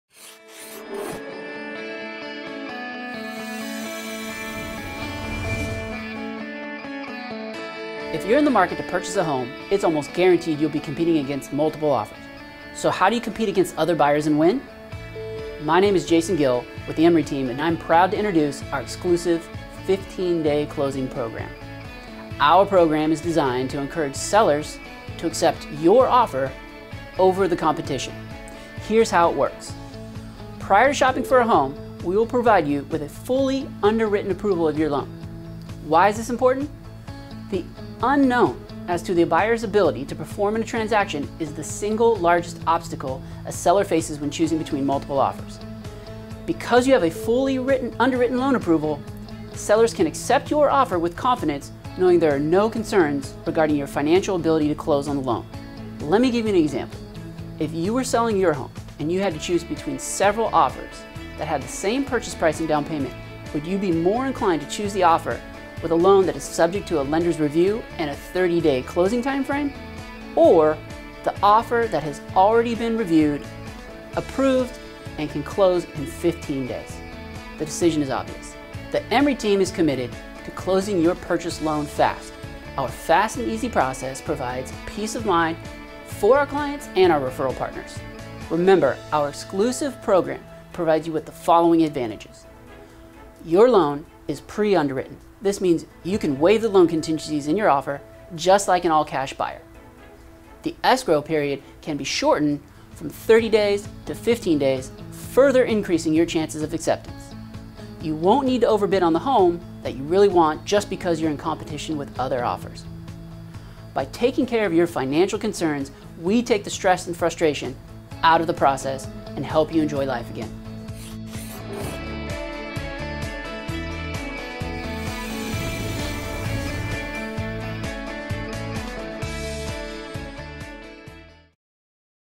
JASON GILL & KELLER WILLIAMS
We go above and beyond the call of duty by providing you with an approval that will stand out from your competitors when in a multiple offer situation.

Watch this short video as Jason Gill explains that the first goal of our team is to help you and your real estate agent get your offer accepted…